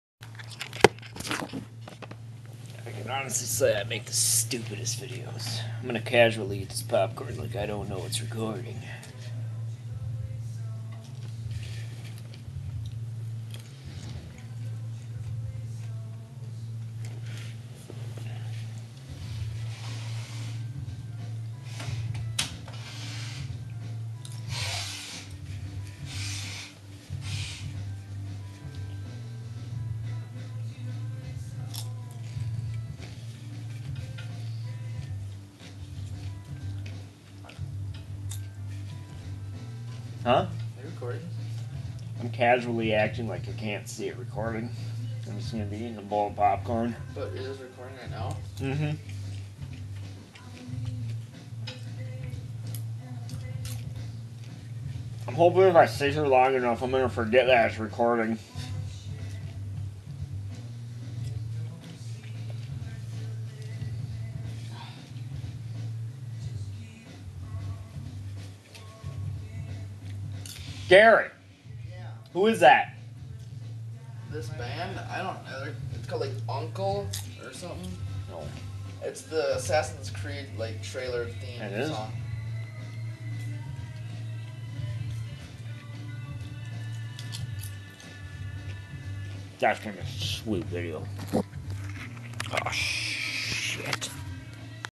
Casually Eating Popcorn
This video honestly has no point, i just wanted something to post for the evening.